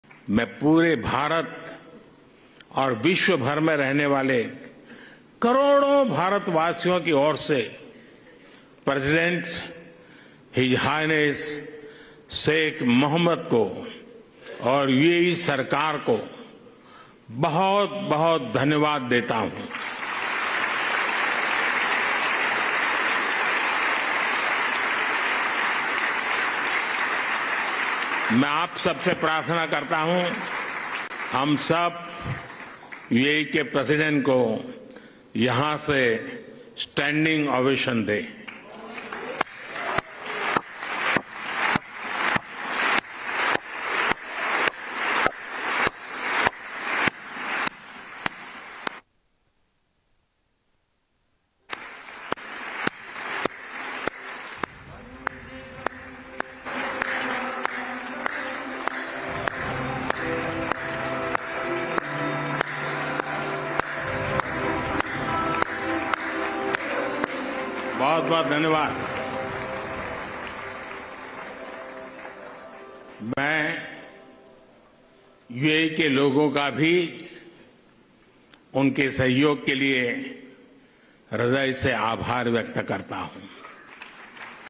PM Modi ने UAE के राष्ट्रपति Mohammed bin Zayed Al Nahyan को दिया धन्यवाद
PM Modi UAE Visit: पीएम मोदी (PM Modi) ने कहा कि UAE के राष्ट्रपति और उनकी सरकार को बहुत बहुत धन्यवाद देता हूं.